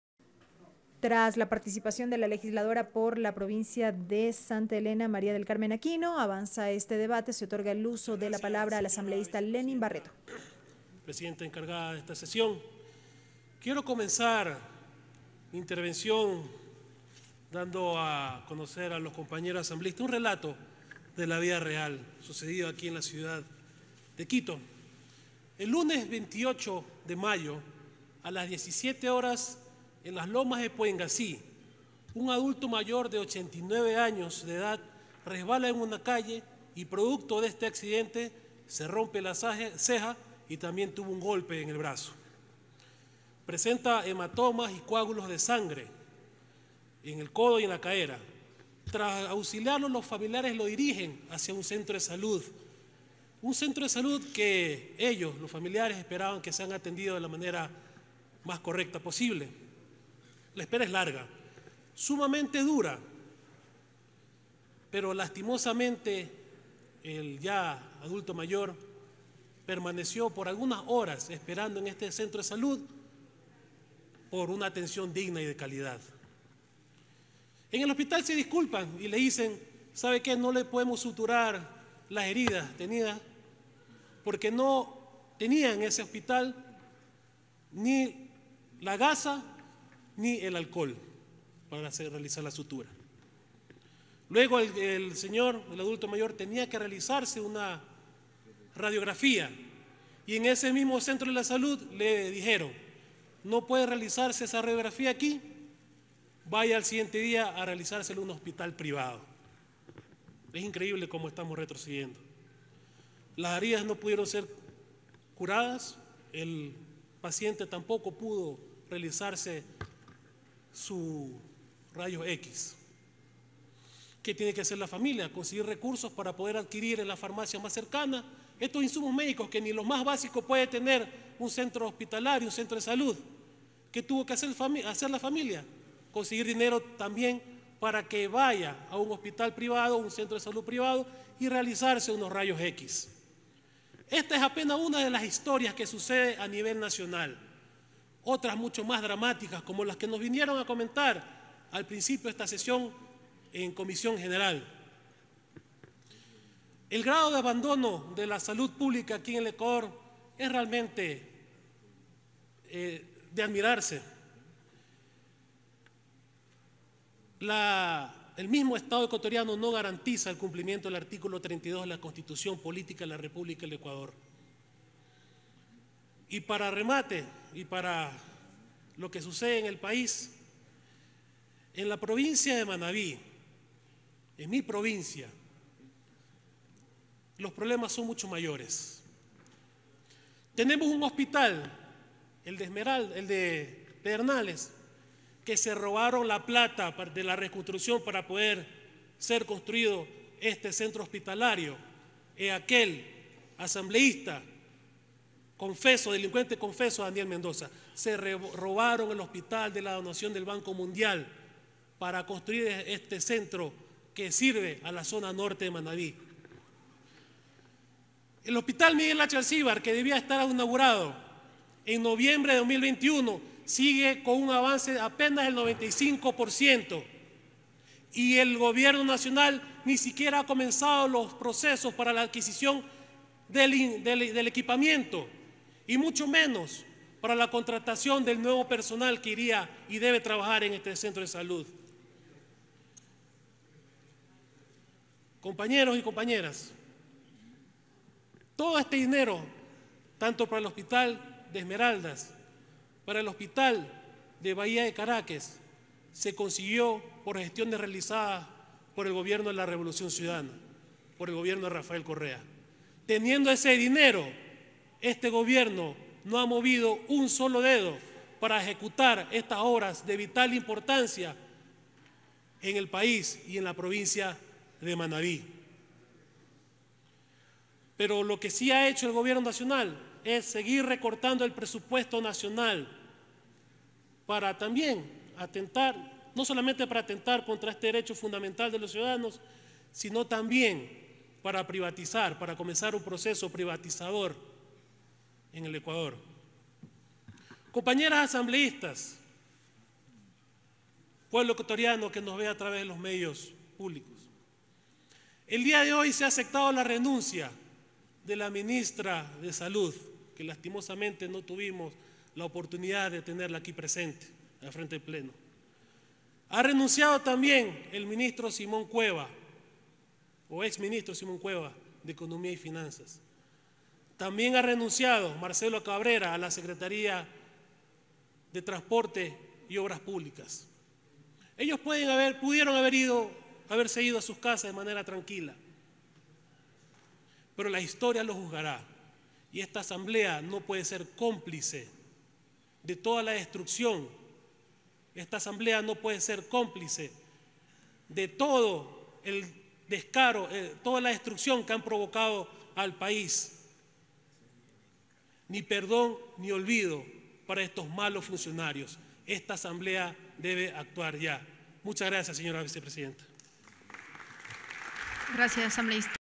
Asambleísta Lenin Barreto - Sesión 784
05 de julio de 2022
PlenoLegislativo | @LeninBarreto relata que un adulto mayor de 89 años de edad luego de sufrir una caída en el sector “Loma de Puengasí”, no pudo ser atendido totalmente en un centro de salud. Reclama el recorte presupuestario en salud.